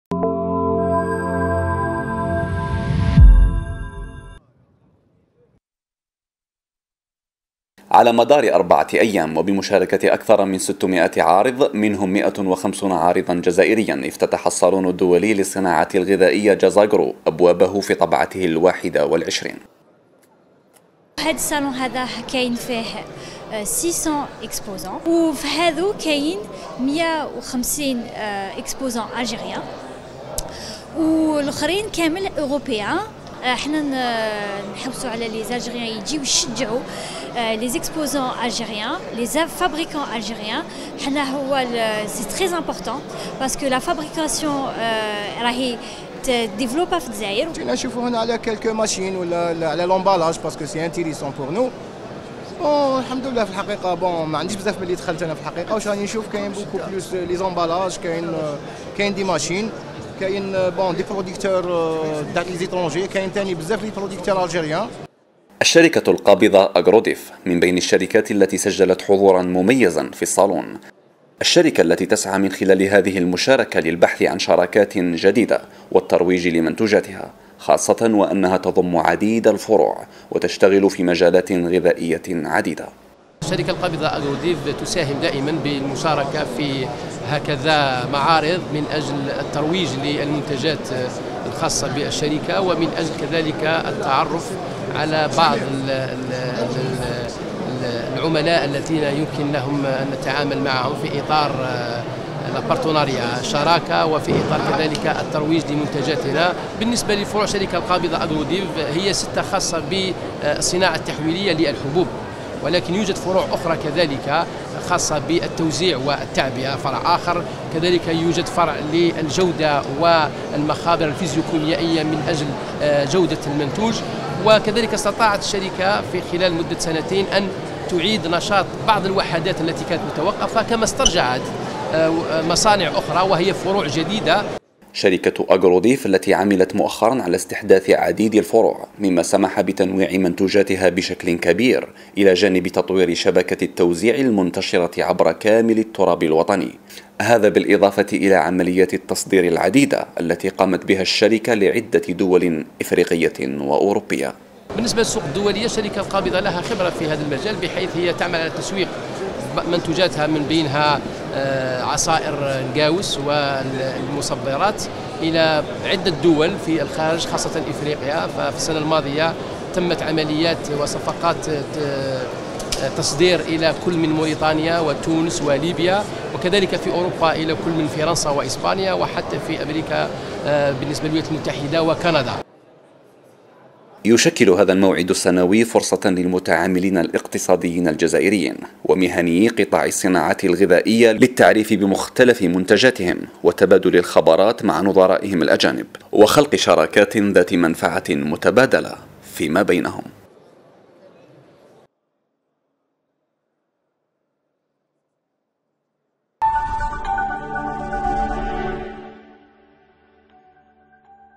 الصالون الدولي للصناعات الغذائية "جازاغرو" : الطبعة الـ21 تحت شعار الابتكار
الجزائر (واج) - يشارك ازيد من 650 عارضا جزائريا و اجنبيا في الطبعة الـ21 من الصالون المهني للصناعات الغذائية "جازاغرو 2024"، الذي انطلقت فعالياته بقصر المعارض بالجزائر العاصمة، حتى يعرض على المهنيين و الجمهور آخر الابتكارات في هذا المجال.